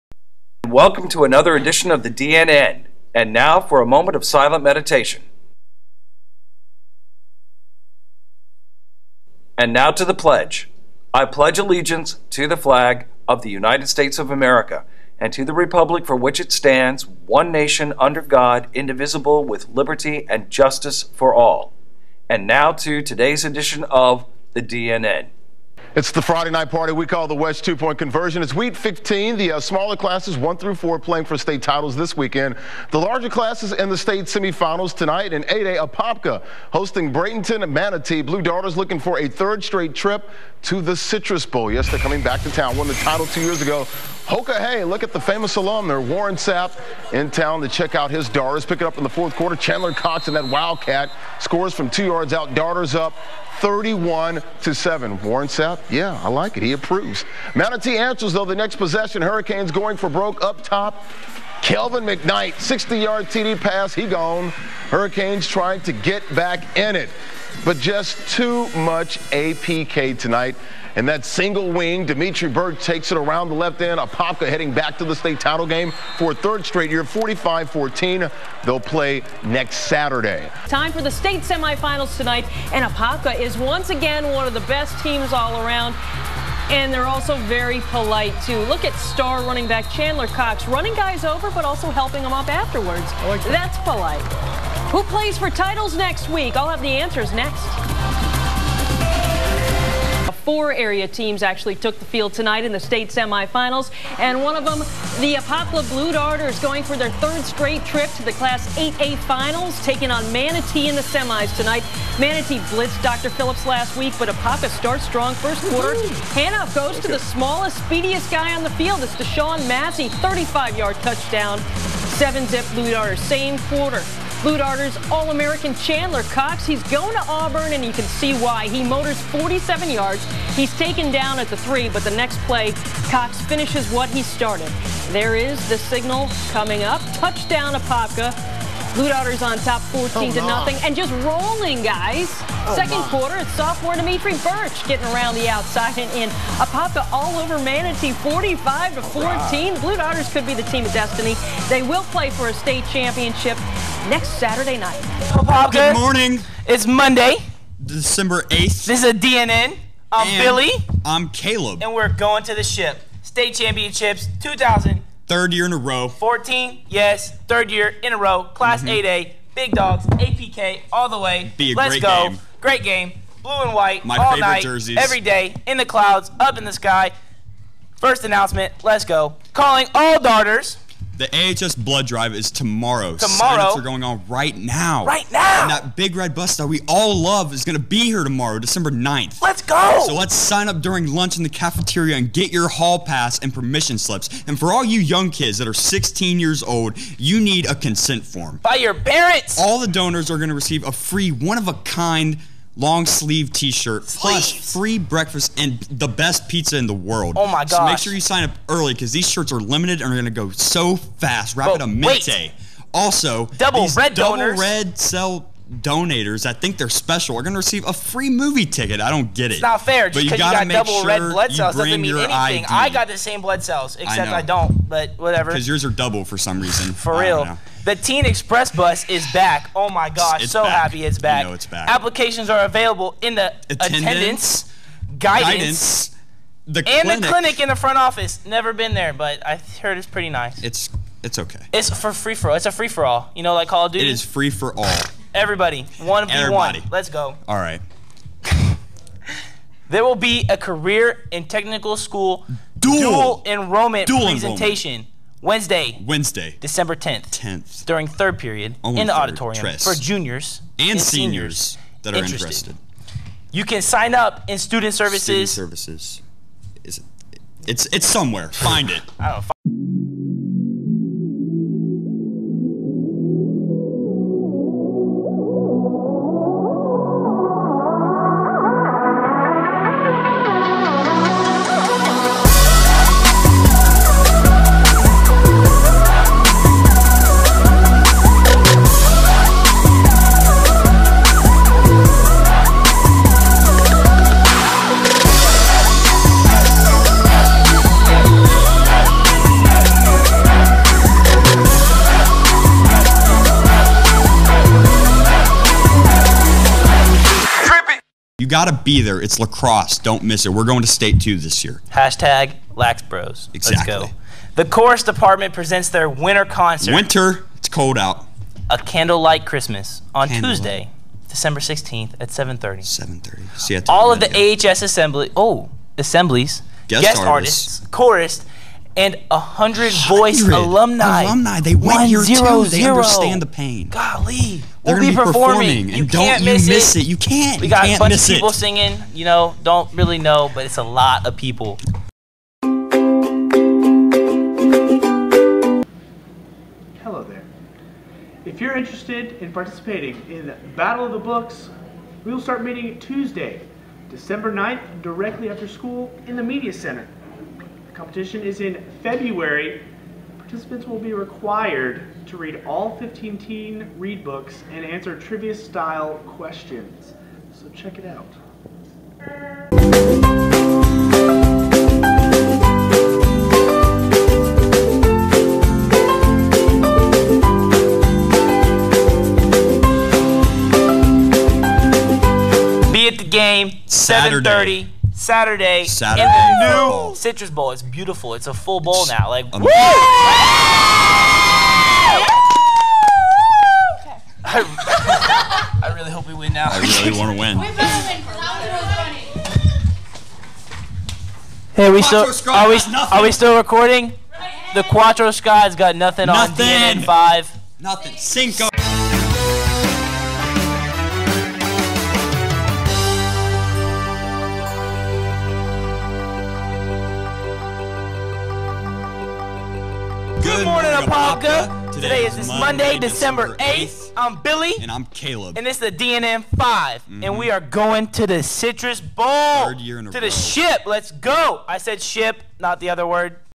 The 2014 DNN: Third Time's A Charm Football Show 12/8/14
The AHS Football is going to state (as of 12/6/14) and the DNN taped two different shows for the day - one where we didn't win and this one - where we did win the game and proceed to State! Highlights from the news and the announcements and commercials show just how great the DNN is for the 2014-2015 school year!